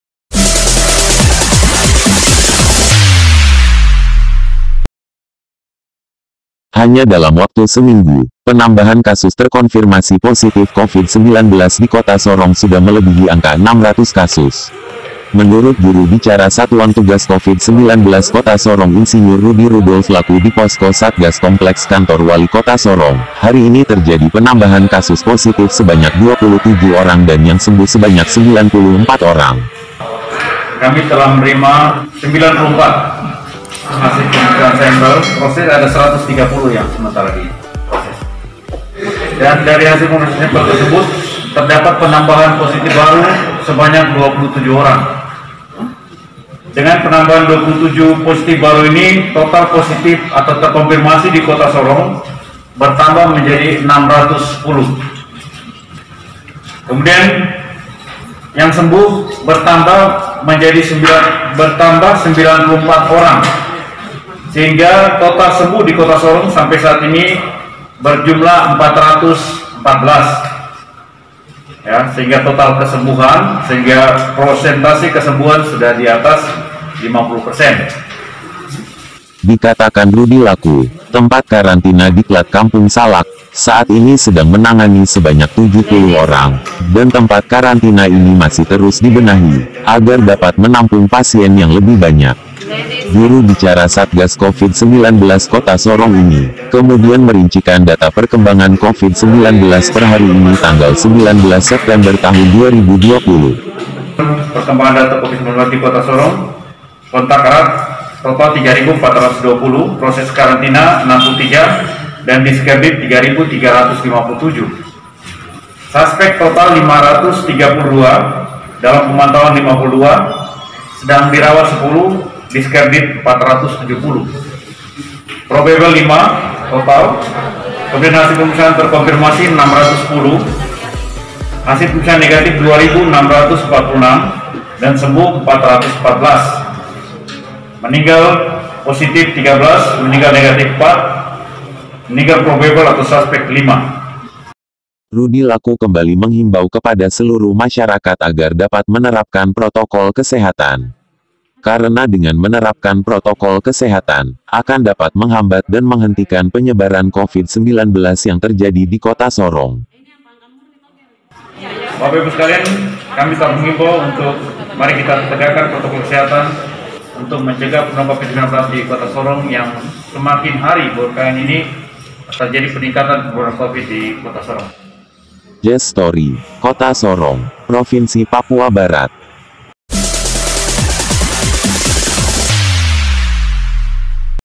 Gawat, Positif Covid-19 Di Kota Sorong Sudah 610 Orang
Ayo Disiplin Menjaga Kesehatan Dengan Cara Menerapkan Protokol Kesehatan.